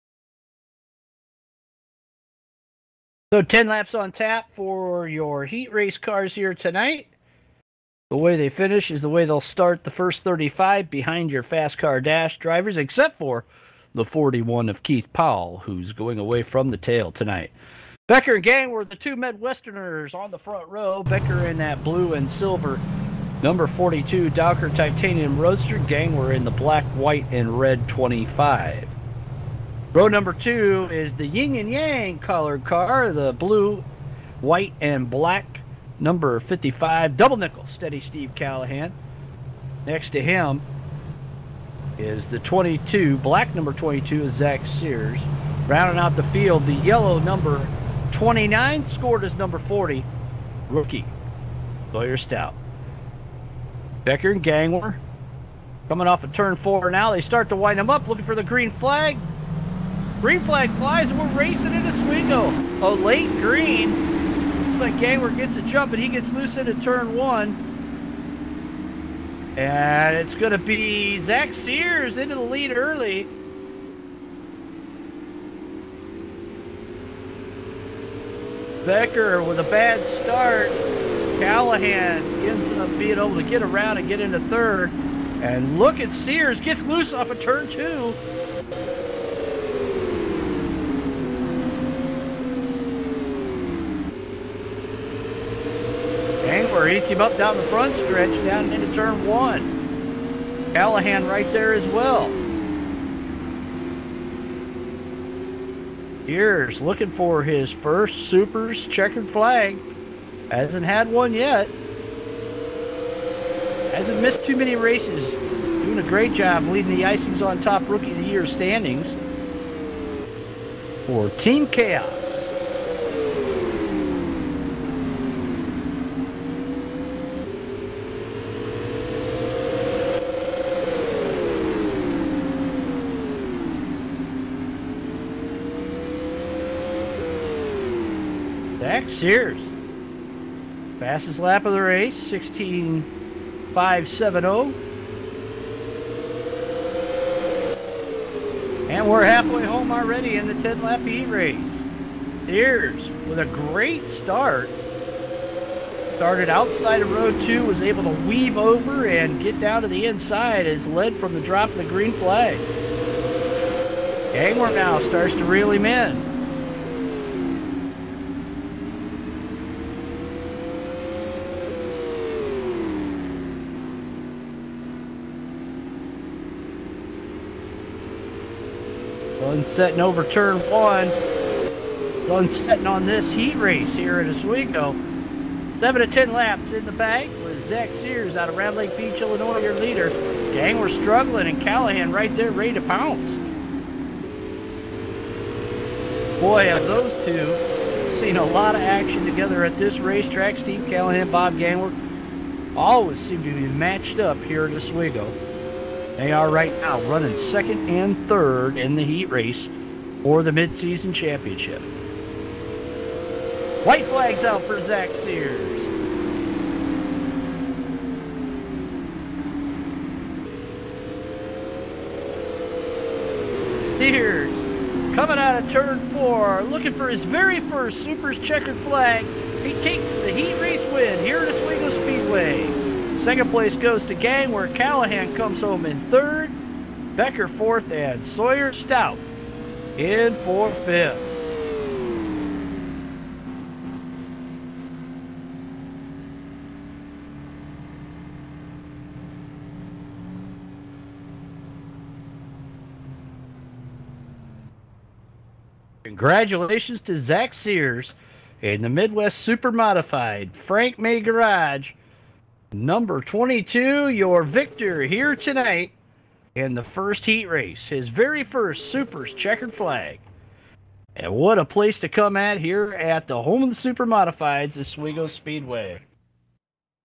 2020 Wing Supermodified Mid-Season Championship Heat Race Oswego Speedway
SUPRS-Icings on Top Rookie of the Year contender, Zach Sears picked up his very first checkered flag in the Heat Race for SUPRS 2020 Mid-Season Championship Twin 35s for winged supermodifieds at Oswego Speedway.